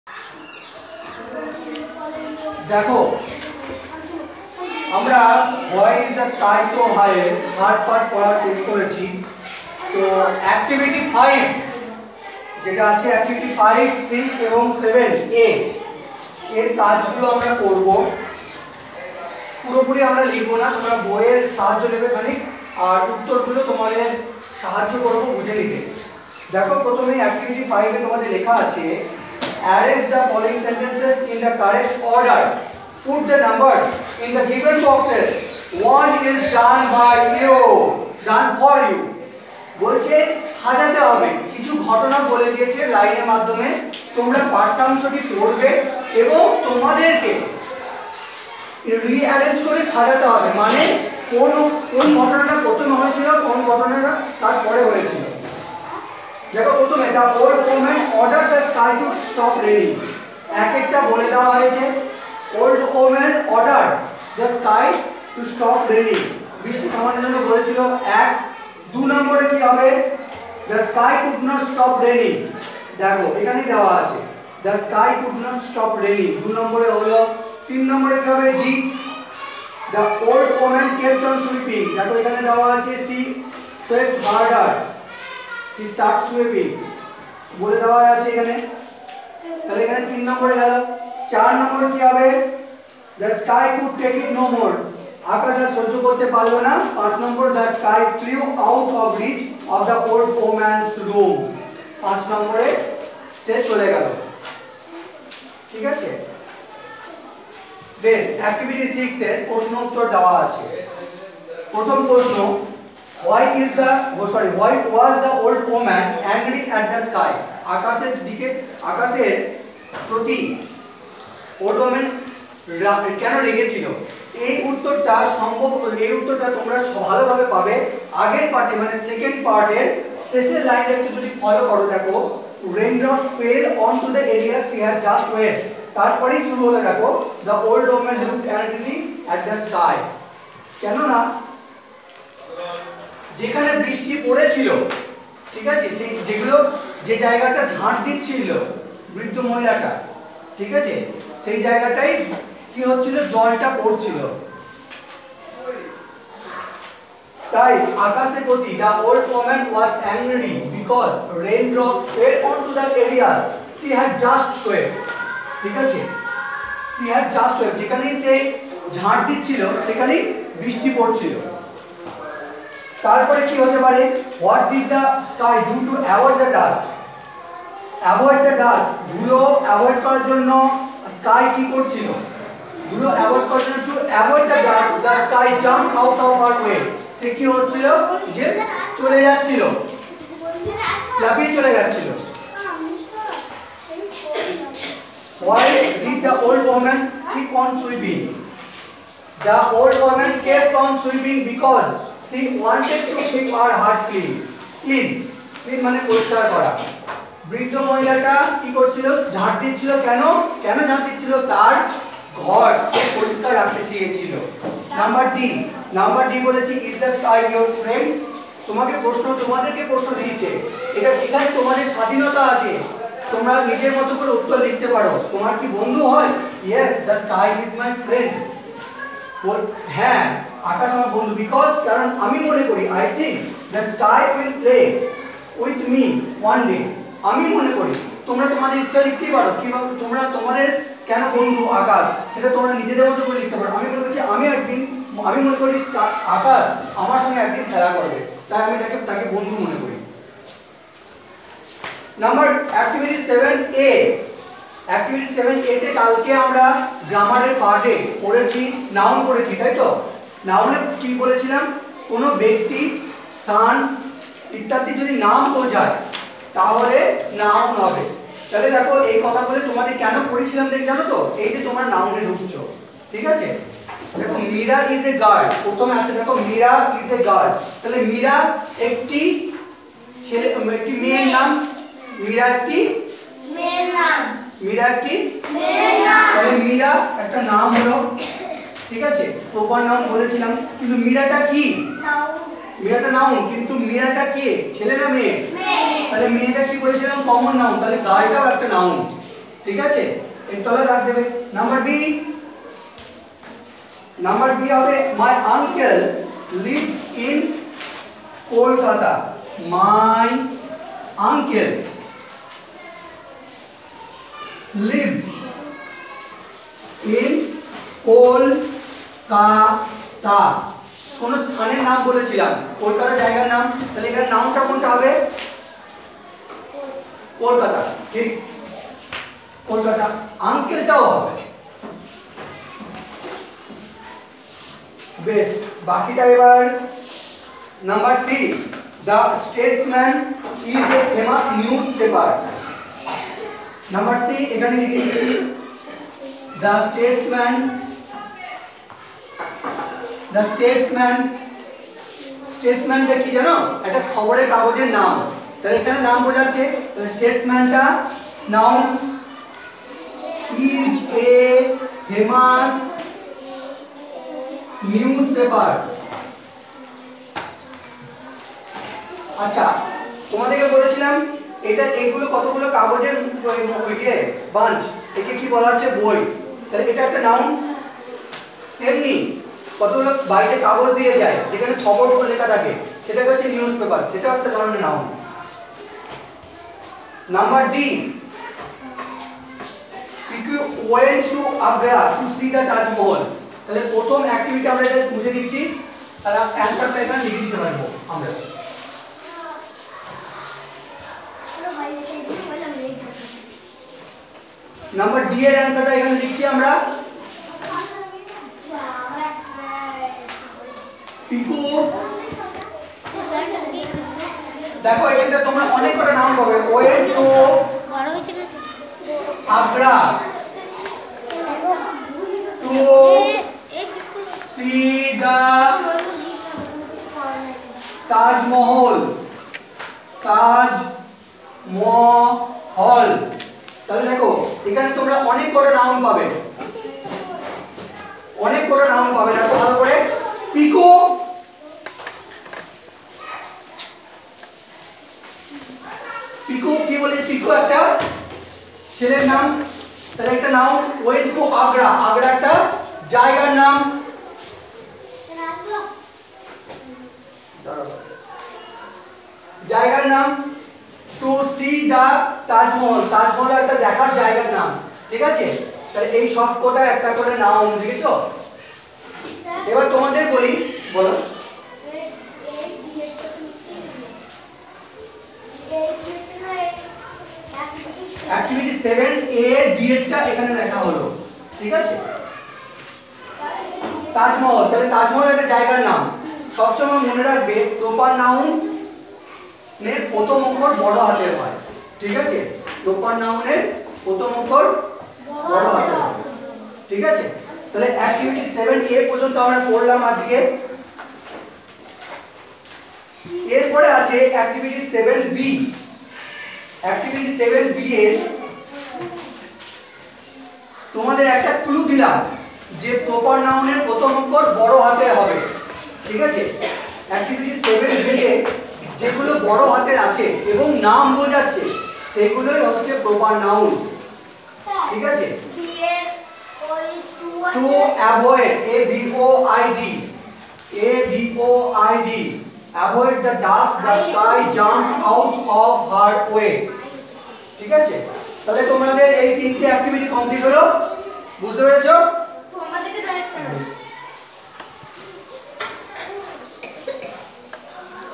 CLASS FOUR||ENGLISH ||WHY THE SKY IS SO HIGH ||ACTIVITY 5-7
Activity - 5

👉 Arrange the following sentences in the correct order. Put the numbers in the given boxes. One is done for you :

(a) The Sky flew out of reach of the old woman's broom. [5]

(b) The sky could take it no more. [4]

(c) The old woman ordered the sky to stop raining. [1]

(d) The old woman kept on sweeping. [3]

(e) The sky could not stop raining. [2]

Activity - 6

👉 Answer the following questions :

(a) Why was the old woman angry at the sky?

Ans. Because, the sky's eyes water fell as drops of rain. Raindrops fell on to the areas she had just swept.

(b) What did the sky do to avoid the dust?

Ans. The sky jumped out of her way.

Ans. Because, She wanted to keep her hut clean.

(d) Is the sky your friend? Give reasons for your answer.

Ans. Yes. The sky is my friend. Because, I play with the sky and the sky fell drops of rain.

Activity - 7 (a)

👉 Circle the words which are the names of persons, places or things :

(a) Mira  is a girl.

(b) My uncle lives Kolkata  .

(d) Piku  went to Agra  to see the Taj Mahal  .